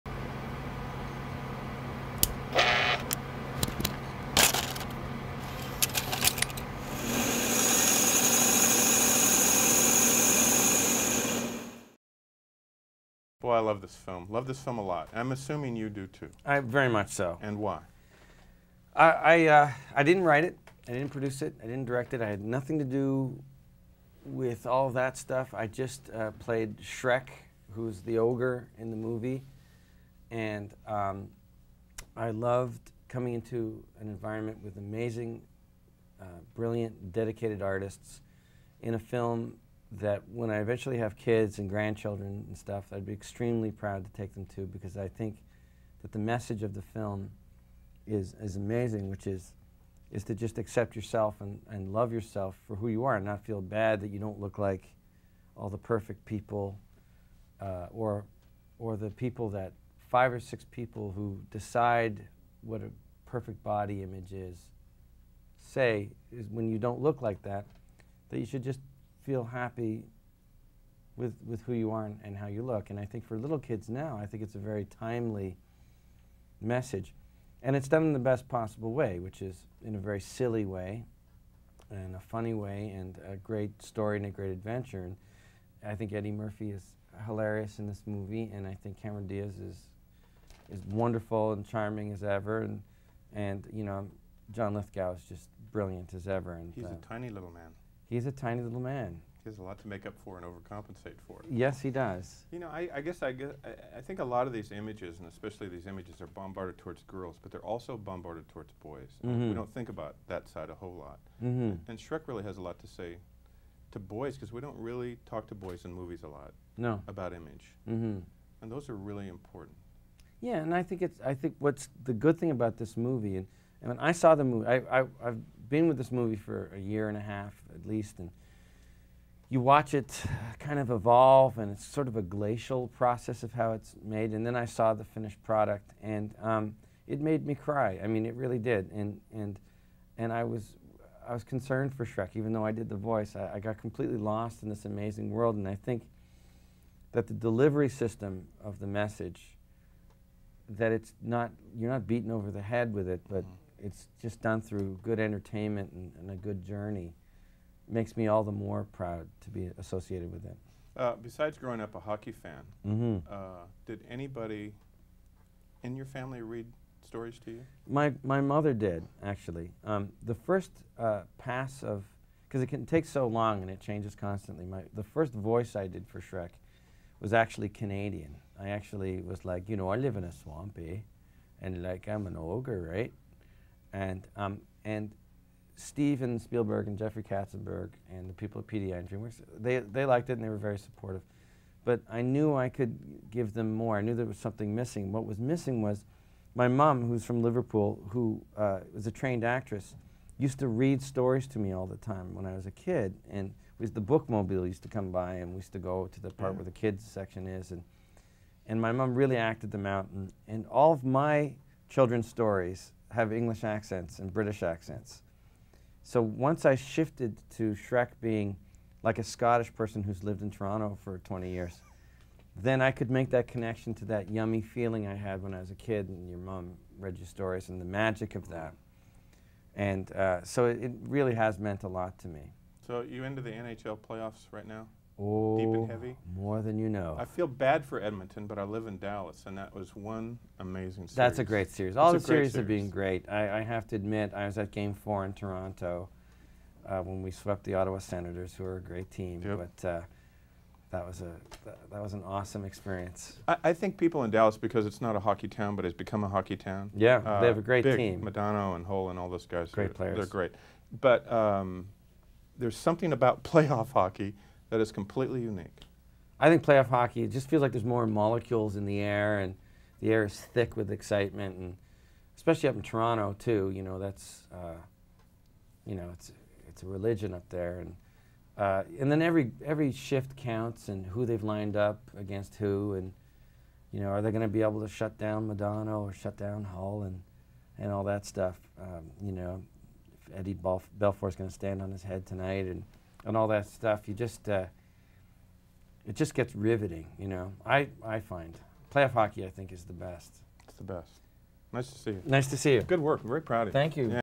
Mike Myers talks about being the voice of Shrek
Mike Myers sat down with WFAA to talk about taking on the role of Shrek in the 2001 film Shrek.